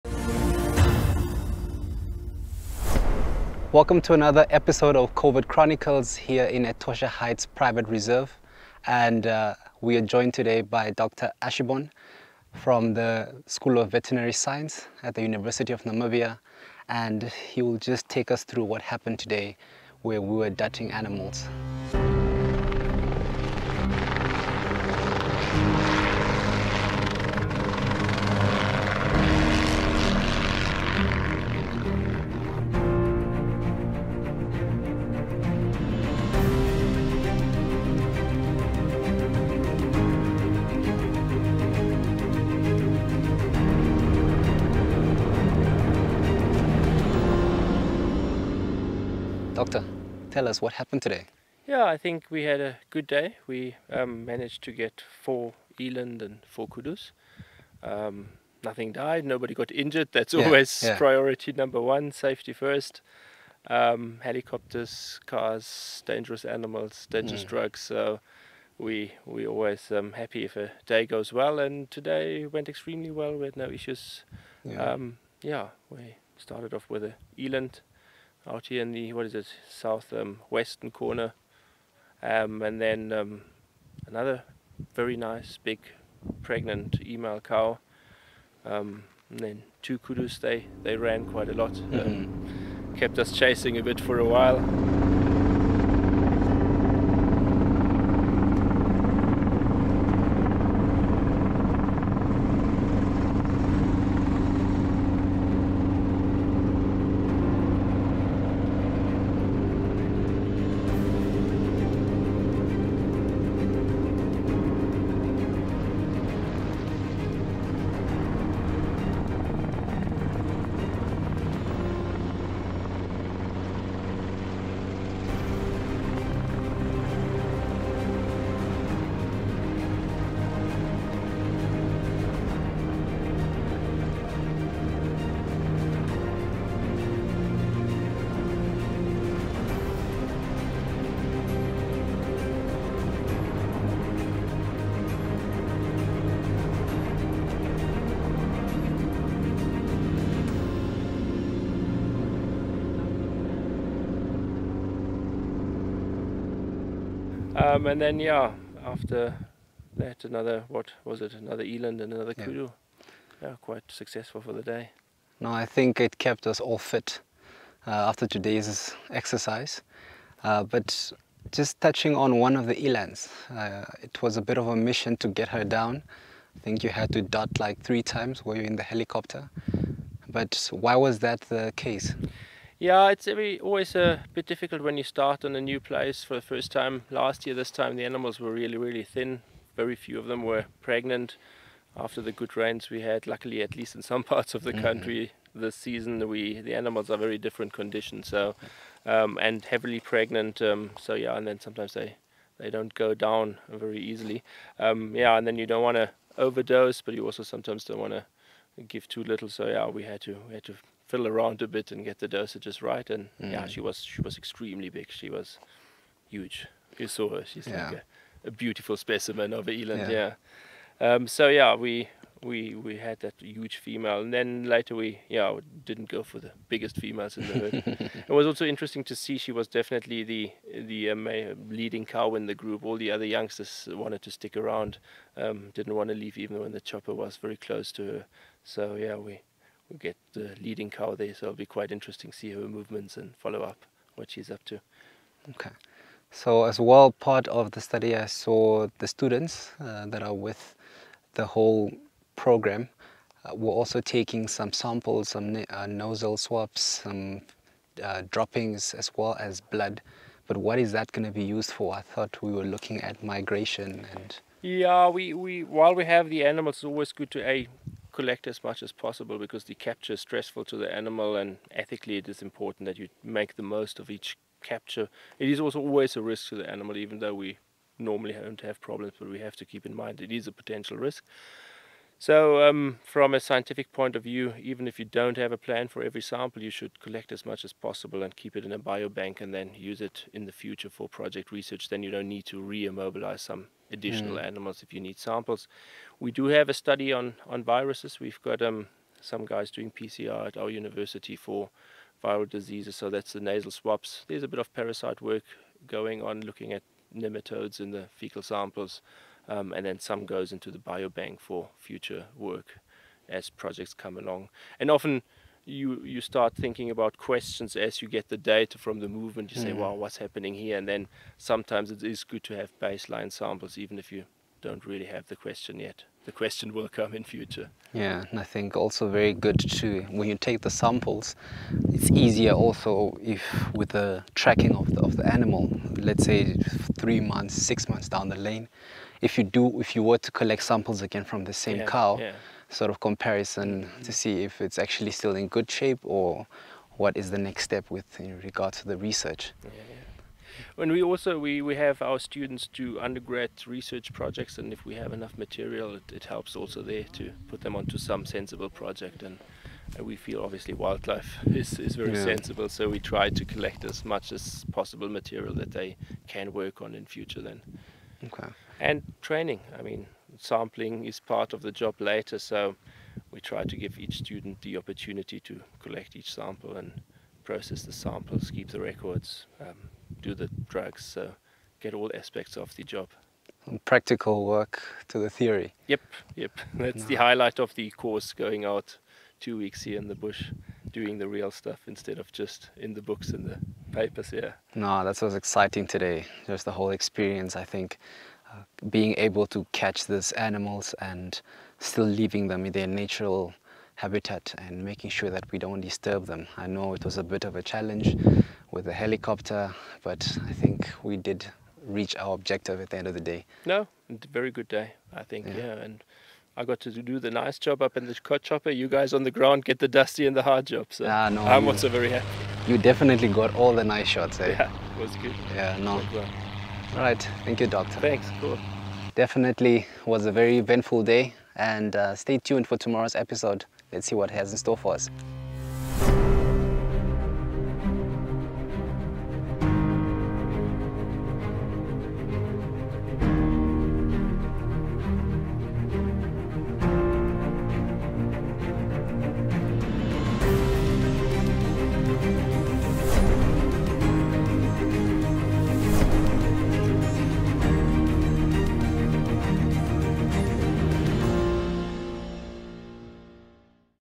E39 VETS IN ACTION
S01: NAMIBIA - CONSERVATION FIRST - COVID CHRONICLES

Episode 39: Vets in Action, NUST Biodiversity Research Centre

Wildlife conservation relies on the support of qualified and specially trained wildlife veterinarians. Both the @NUSTBRC and @UNAM took their final year students to the NUST Biodiversity Research Centre at Etosha Heights Private Game Reserve.  This action filled episode showcases one day of intense adventure with Dr Ortwin Aschenborn and Dr Mark Jago who show the students what it is like to be a wildlife veterinarian.

Hold on to your seats for this virtual look into an exciting mix of wildlife clinical studies and an ecological long term study for Namibia by Namibians.